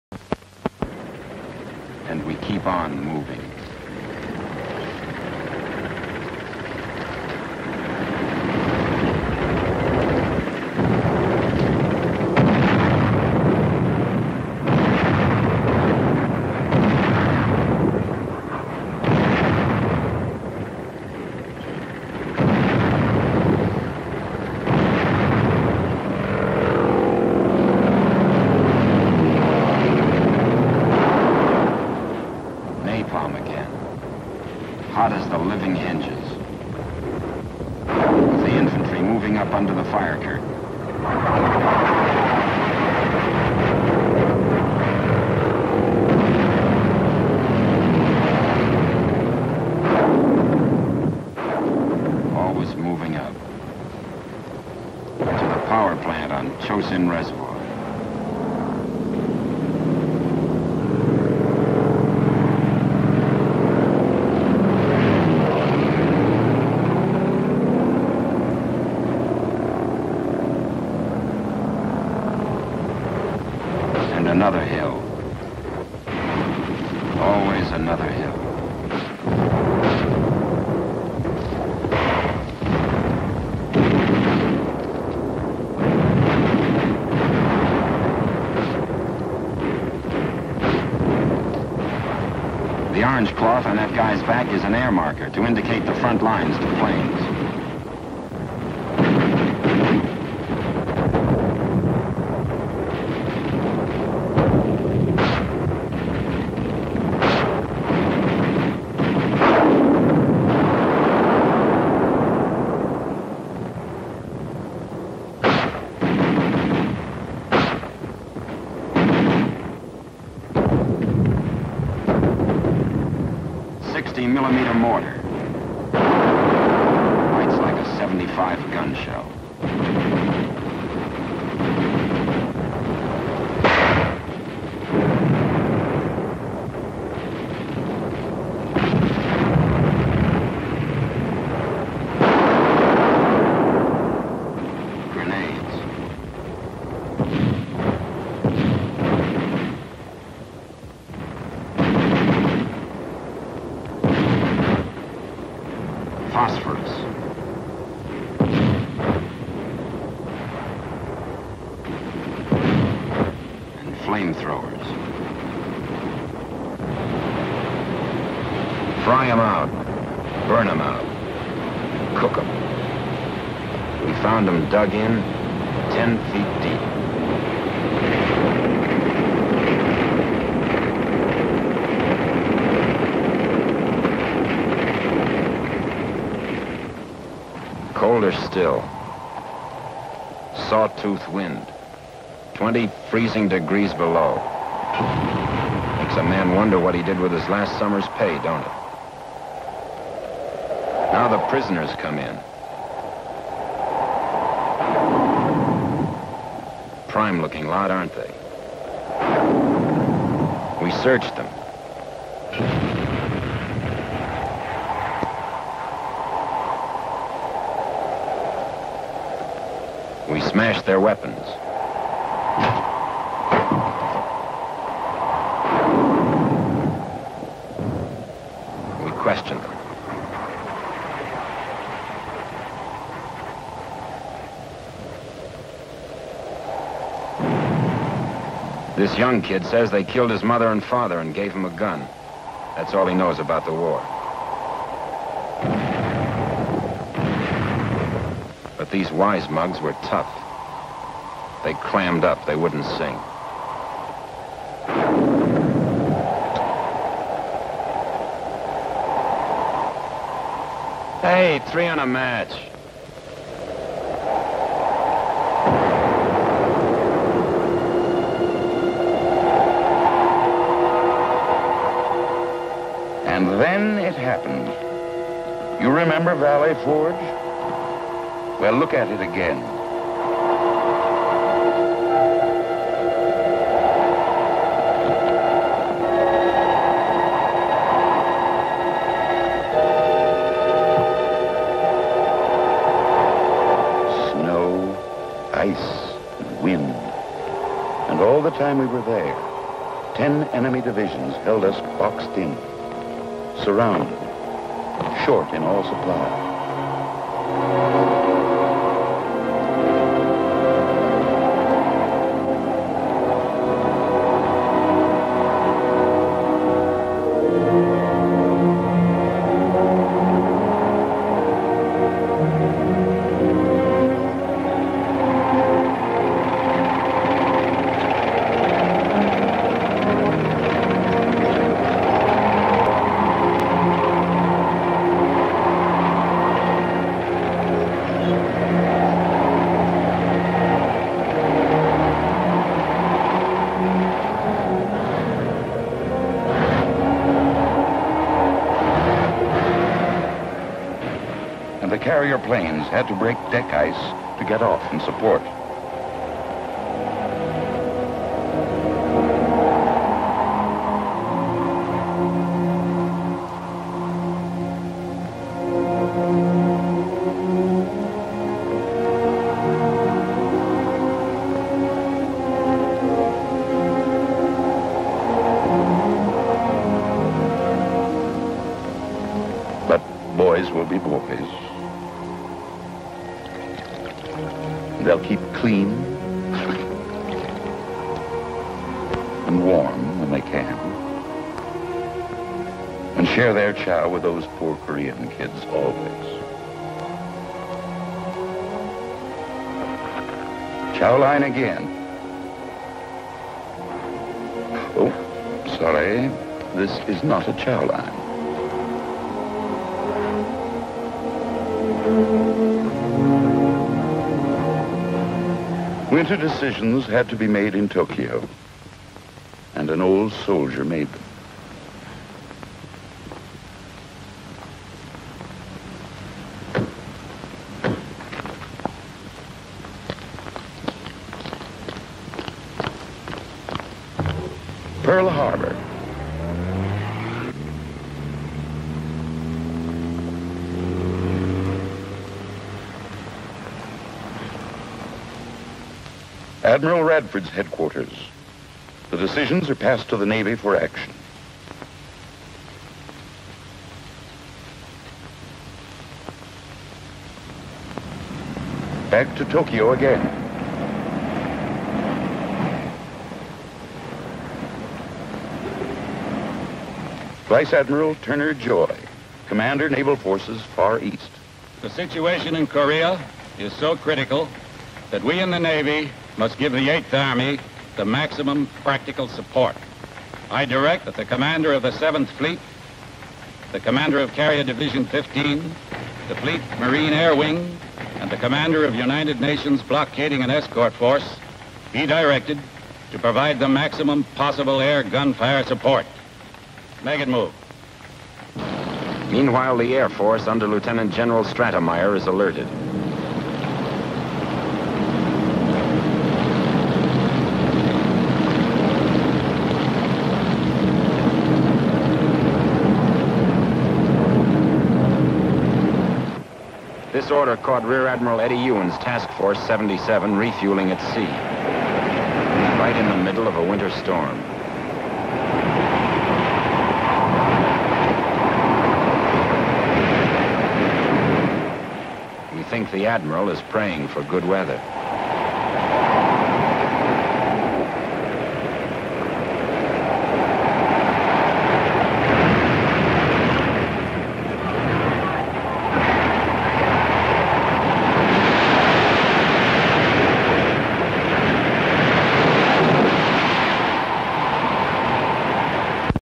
Korean War Documentary - 250062-04 | Footage Farm Ltd
R4 of 6  Color, Documentary Film re How Korea became embroiled in Cold War politics & story of 7th Fleet & 1st Marine Division in Korean War in first year.
LS across valley as Marines move up on foot, w/ tanks, artillery firing; planes strafing w/ napalm.  Infantry up w/ strafing beyond.
02:26:29  Aerial over power plant on Chosin Reservoir.
02:26:48  Marines w/ packs up steep, rocky hillside.
02:27:08  Firing automatic rifle from sandbagged position; others firing sniper rifles & bazookas.  Many different intercut (some scenes have patches of snow).  View down onto lower ridge hit w/ mortar rounds.  Grenades thrown down; phosphorous exploding.  Flame throwers along ridge.
02:28:42  Men climbing up snowy ridge towards camera; taking prisoners, searching, breaking weapons along cliff.
02:29:23  MCU  Questioning beside fire at night thru interpreter.
02:30:01  Pan across artillery & crates in snow, tents in background to jeeps & trucks; men getting food outside in blowing snow / blizzard.  Men on parked tanks.  Tank moving in snow past other equipment.
02:31:26  Snow covered fighter jets w/ wings folded up on aircraft carrier w/ ice & snow on deck as snow falls.
02:31:26  Pan snow covered equipment w/ tents behind.  Several soldiers having snowball fight; wash in bucket; standing around fire w/ Korean kids, CU.  Men wait in snow for outdoor toilet.
02:32:30  Tokyo, Japan.  Limousine arrives, General Douglas MacArthur out & past camera into building.
02:32:50  Pearl Harbor, Honolulu, Hawaii w/ military men & women along sidewalk.  CU sign:  Commander In Chief Pacific & United States Pacific Fleet.
02:33:00  Admiral Arthur W. Radford at desk, CU of pen signing his name on military form.
02:33:13  Tokyo; sign on awning:  Commander Naval Forces Far East.  Interior meeting w/ Vice-Admiral Turner Joy standing, speaking SOF, two officers listening, male secretary typing:  “The situation in Korea is so critical that we in the Navy must give give the 8th Army the maximum practical support.  I direct that the commander of the Seventh Fleet, the Commander of Carrier Division 15, the Fleet Marine Air Wing, & the Commander of United Nations’ Blockading & Air Force be directed to provide the maximum  possible air gunfire support.”  CU of recording typewriter.
02:33:53  CU signs in rain:  Far East Air Forces; 315th Air Division Combat Cargo; 5th Fighter Command; 5th Bomber Command; jeep w/ sign 5th Air Force,
02:34:11  Two Naval ships of Task Force 77 at sea, USS Eversole (DD-789) refueling in winter storm.  CUs; man in bosun chair as waves break.  GOOD.
Anti-Communist; Communism; American Korean War Propaganda; Cold War; Fighting; Battles; 1950; 1951;
NOTE:  Battle of Chosin aka Changjin Lake campaign was 17 days :  27Nov-13Dec50